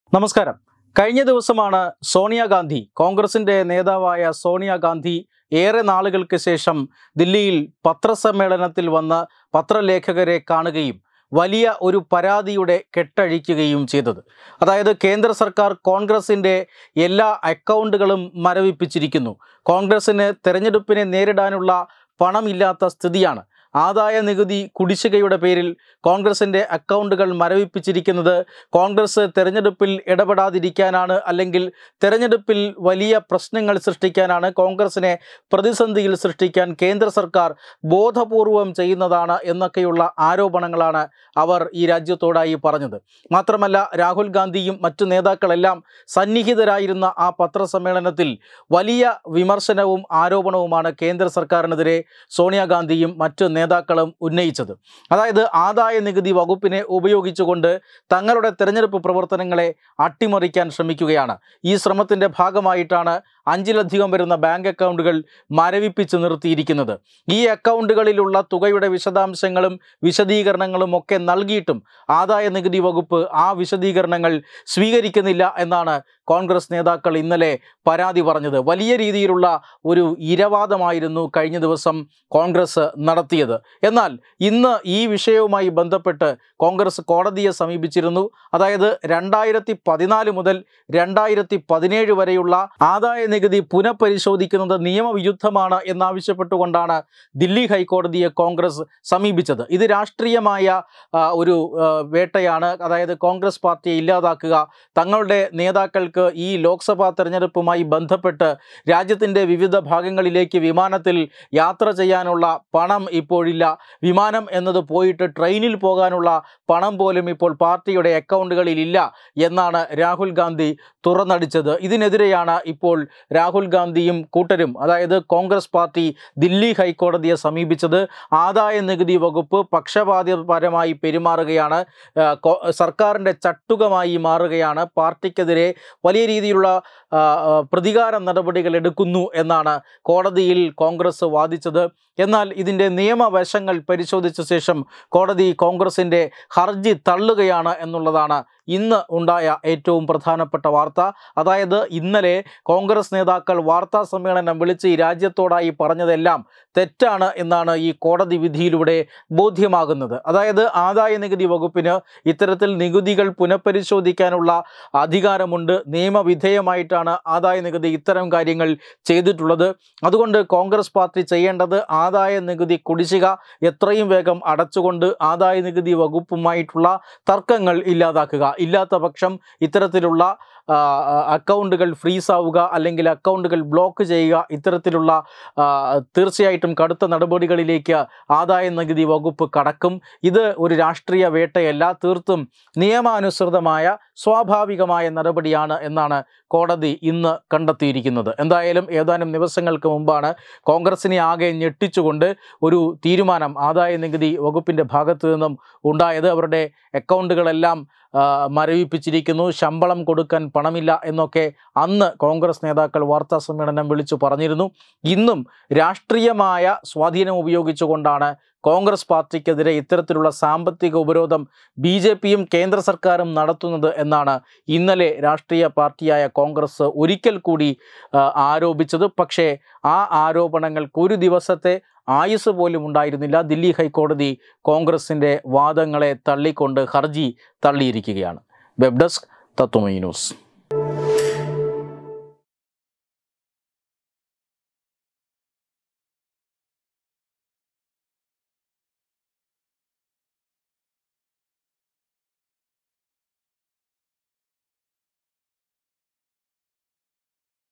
ആദായനികുതി വകുപ്പ് നടപടി നിയമാനുസൃതമെന്ന് കോടതി ! |CONGRESS
കോൺഗ്രസിന്റെ ഇരവാദത്തിന് 24 മണിക്കൂർ പോലും ആയുസുണ്ടായിരുന്നില്ല |CONGRESS|